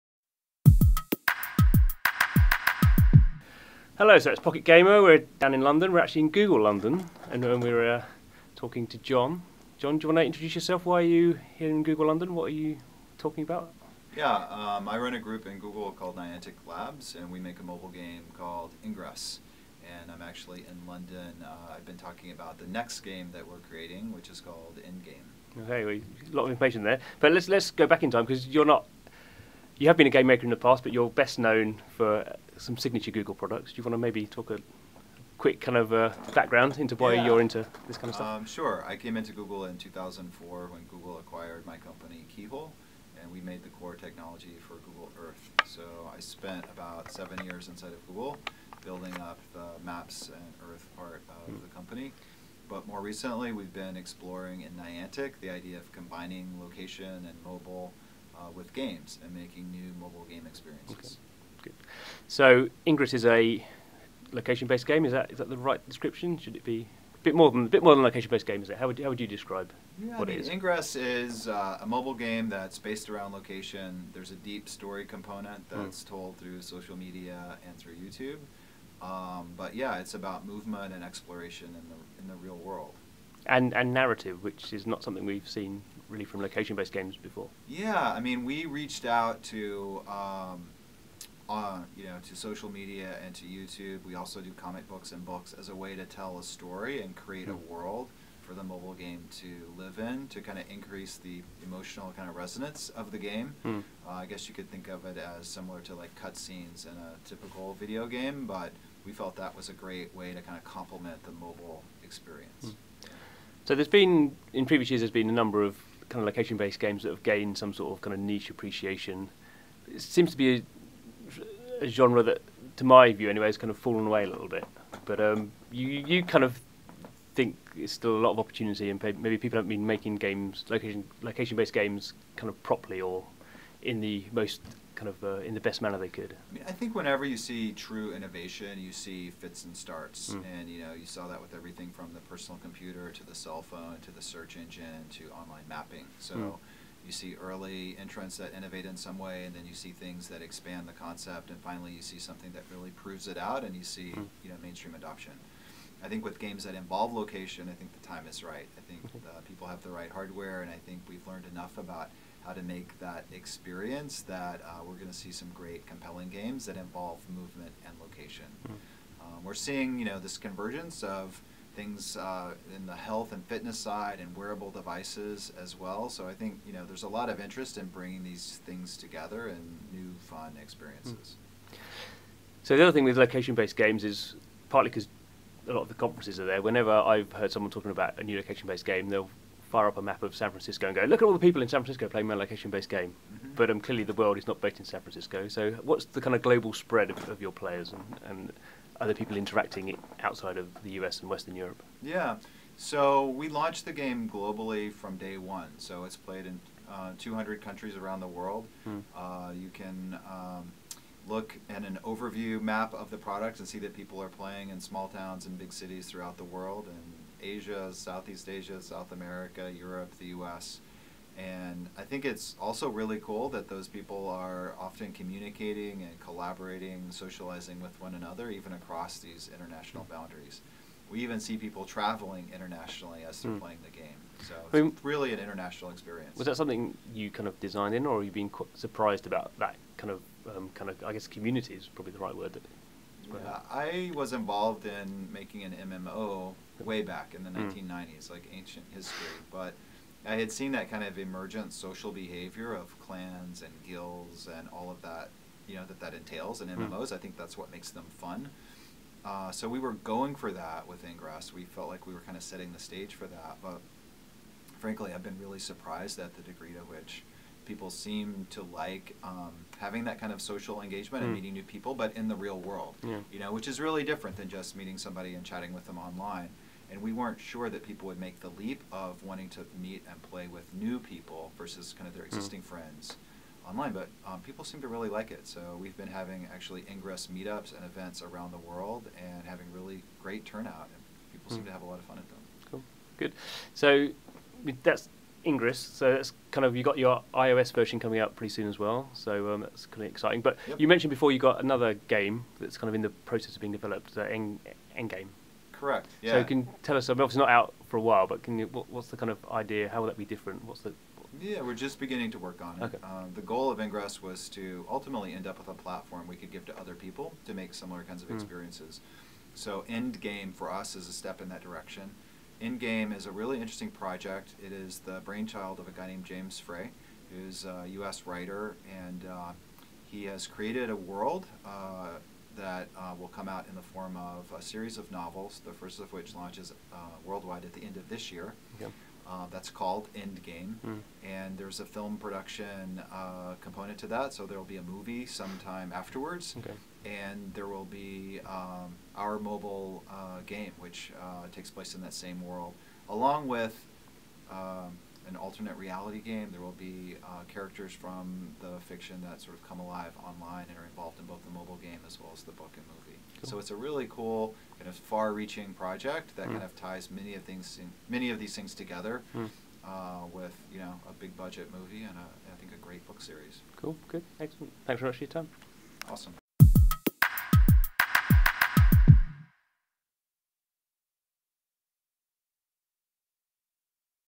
John Hanke on why location unlocks the next wave of mobile gaming
Google Earth guy, now CEO of location-based game developer Niantic Labs, John Hanke knows location is important. In this interview, he talks about the social and emotion power of Niantic's game Ingress, as well as the studio's future project Endgame.